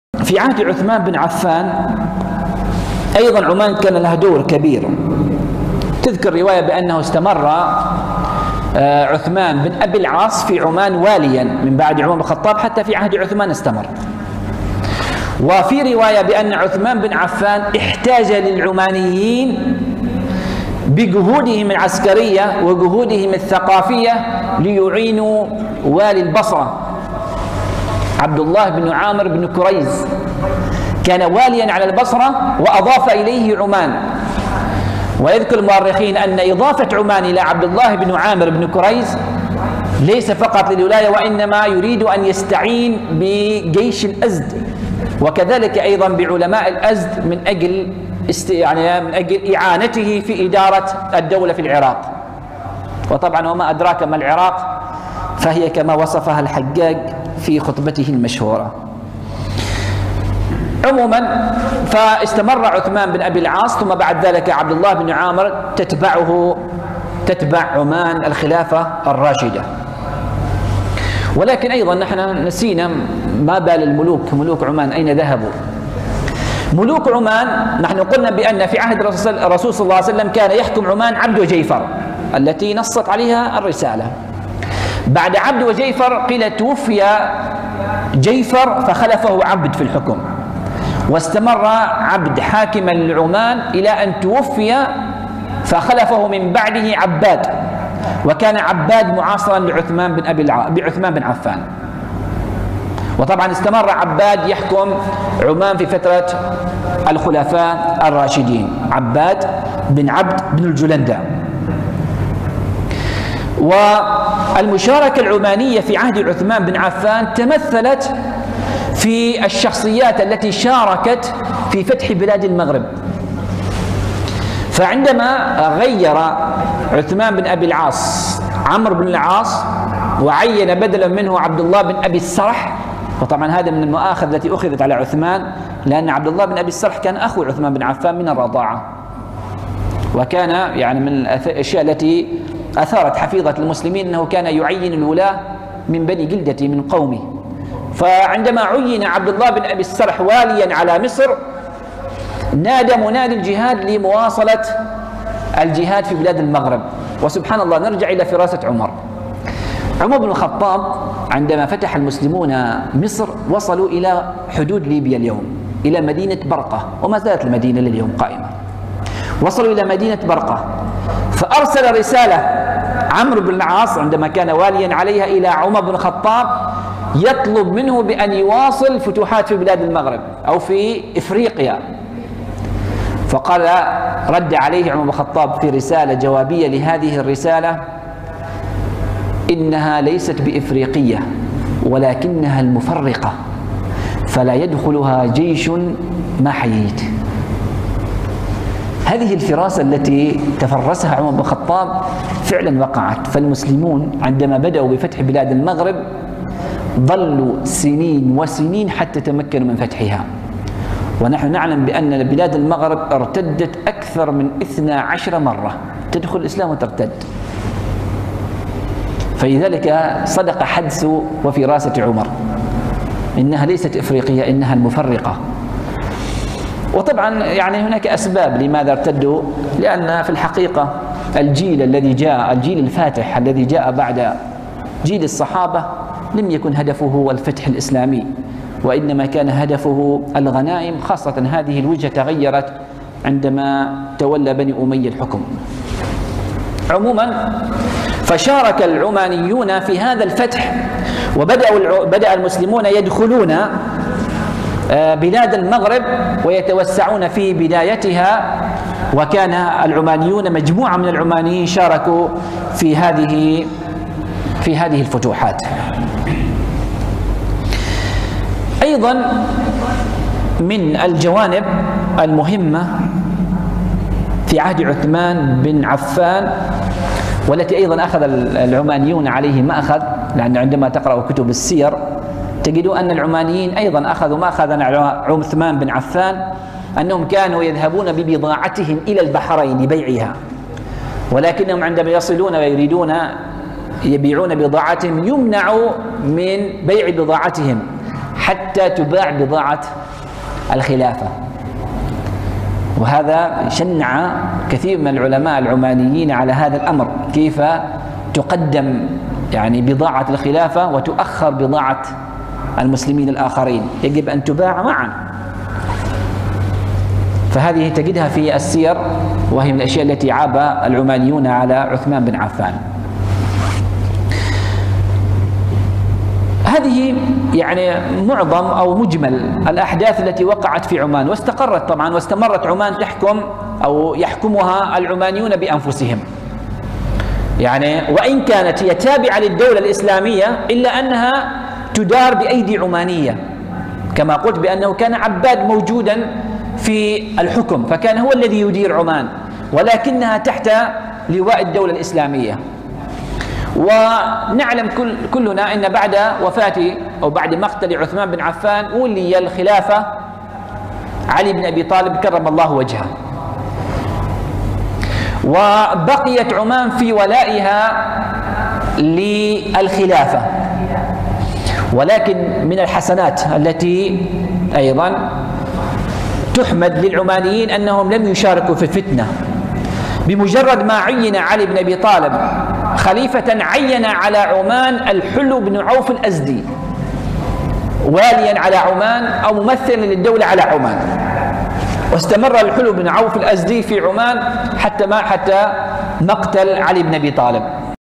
العمانيون في عهد الخليفه عثمان بن عفان رضي الله عنه. للدكتور ناصر الندابي
مصدر المقطع:

سلسلة دروس في التاريخ العماني 2

للدكتور ناصر بن علي الندابي